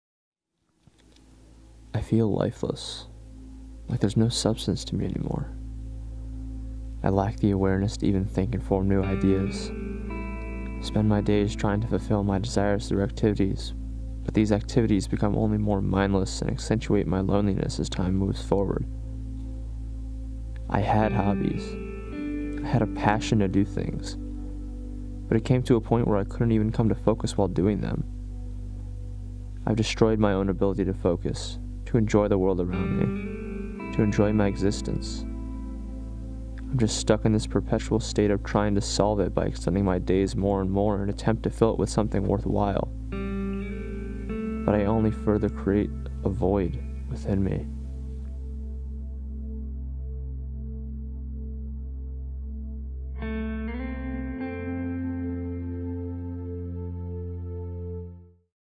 Sleep Deprivation PSA
This is a short video I shot, directed, edited and starred in for a class during my freshman year at Syracuse University.